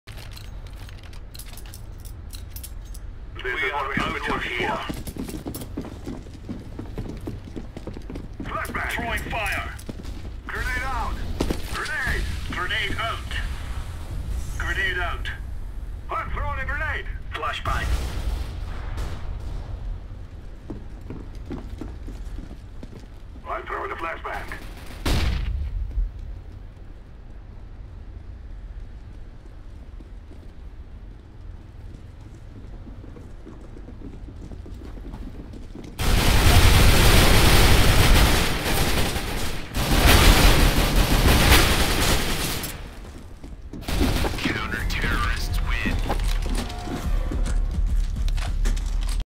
One round with [The*Pun!$her] (testing fraps)
first time trying some fraps recording and youtube upload, i would like to make some short frag movie later :)